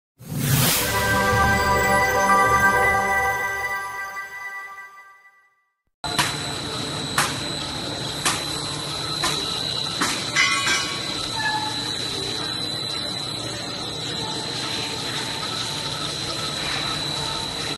pasta / macaroni making machine
pasta making machine
Capacity 100-150kg/h
Change different dies can produce different shapes pasta